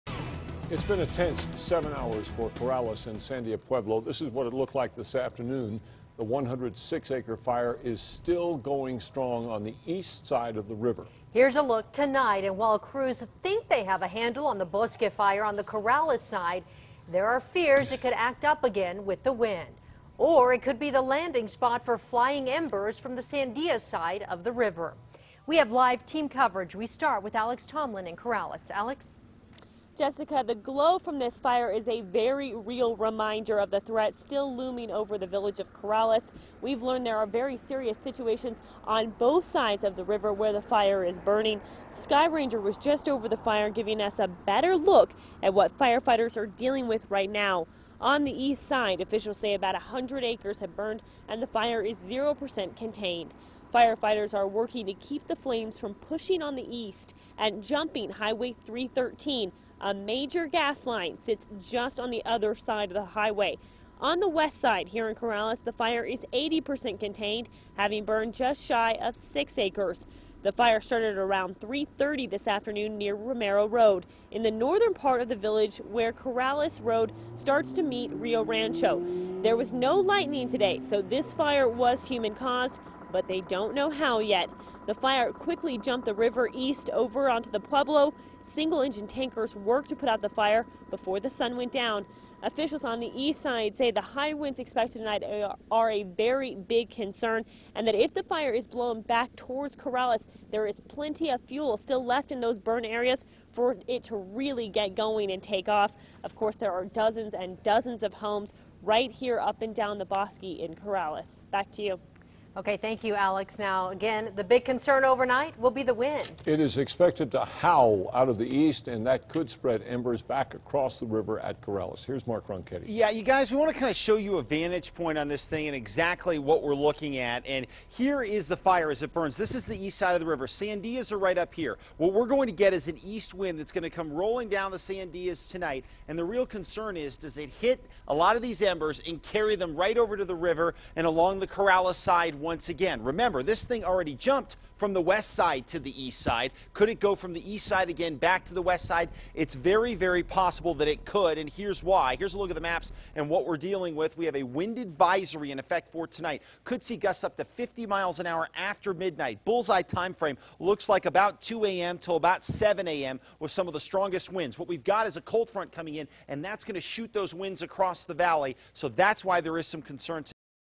Intense fire in Bosque have crews out overnight
Romero fire activity has lessened as the fire is laying down with the cool of the evening setting in. However, crews are anticipating a shift in winds overnight, which could cause the fire to pick up somewhat. Smoke will be present and low to the ground overnight.